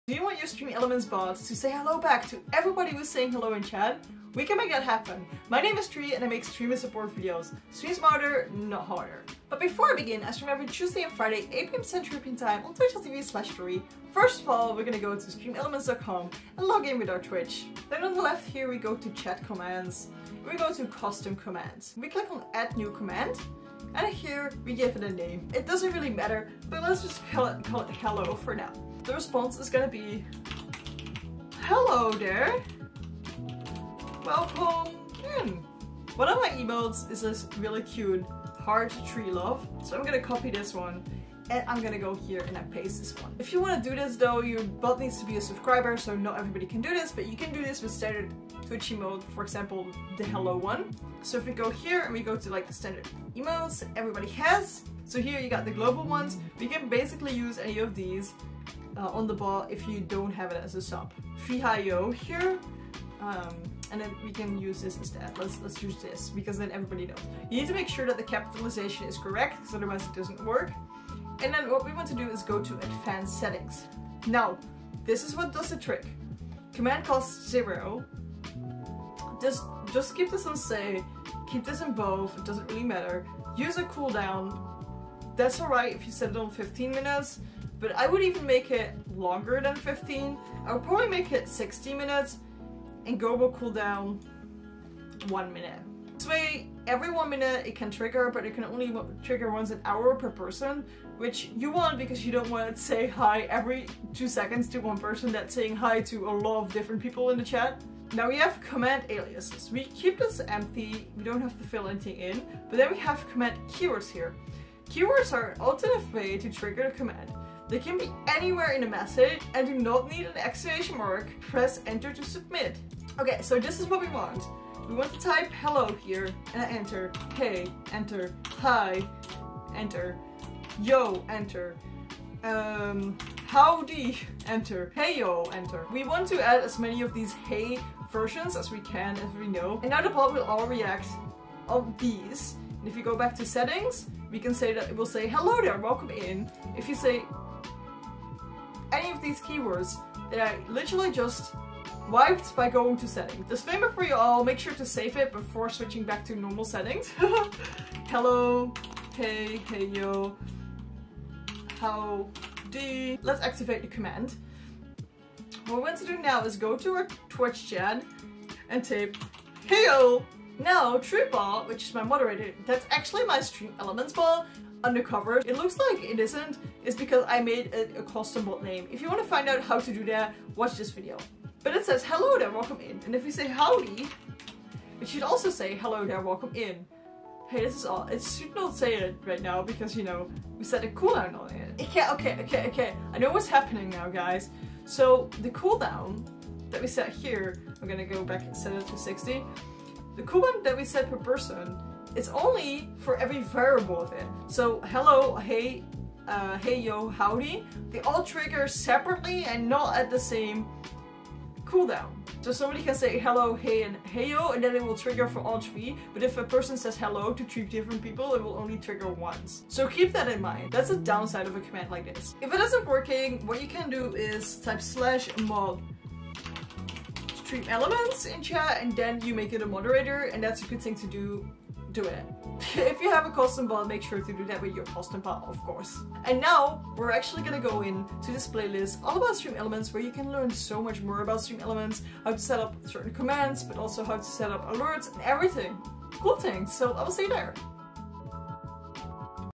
StreamElements Welcome Message
Learn how to let StreamElements say hello to everybody in chat who says hello. You can do this with this StreamElements Welcome Message Tutorial! Trui will go over it and teach you to never miss a hello in your Twitch chat!